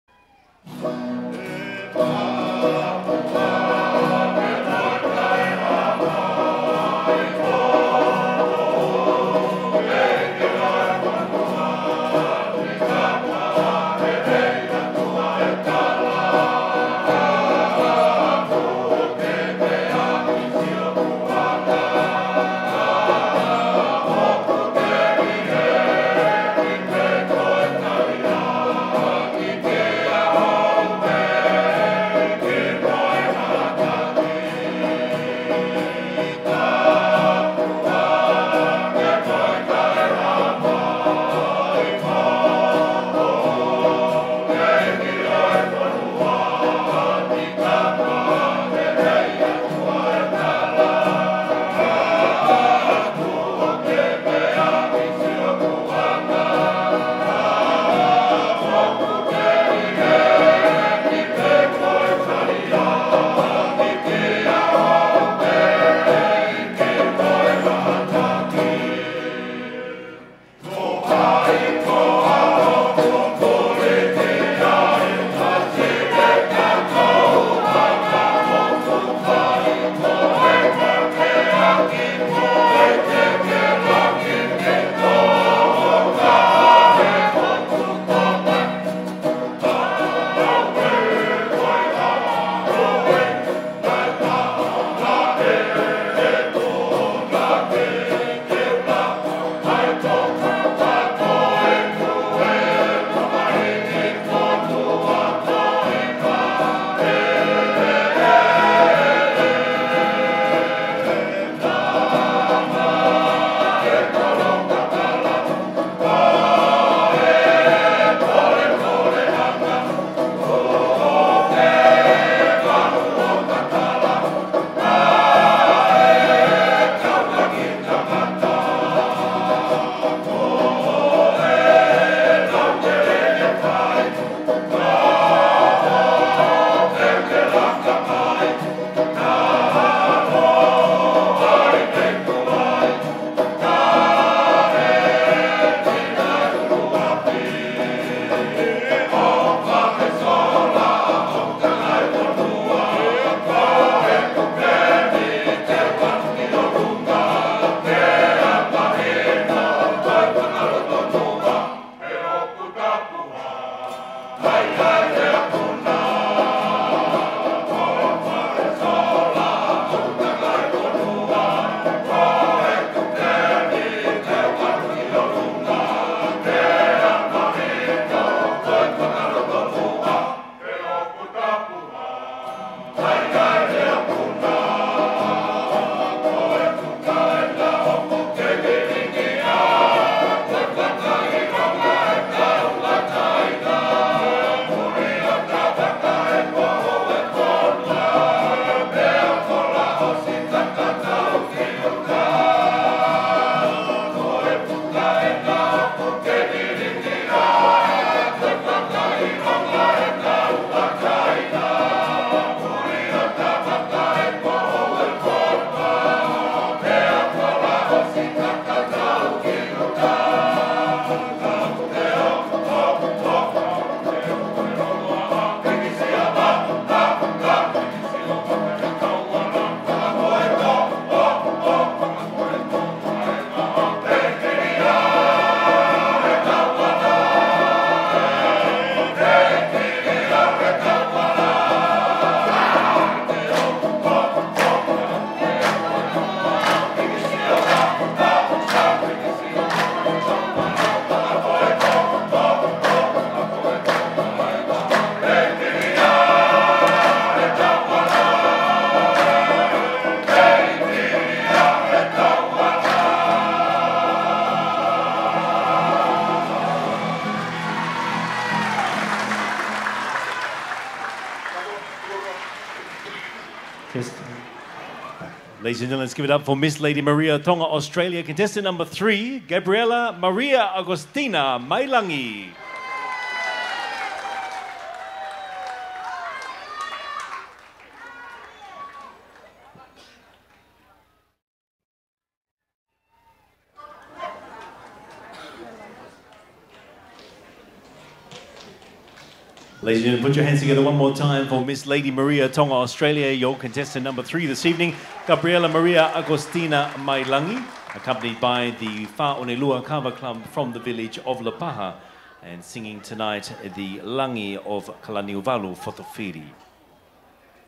🇹🇴 Miss Lady Maria Tonga Australia 🇦🇺 Gabriella Maria Augustina Mailangi 🌺 Miss Heilala Tau’olunga
🌺 Heilala Village, Nuku'alofa, Tongatapu, Kingdom of Tonga. 3rd July, 2024.

Filmed by Broadcom Broadcasting 87.5FM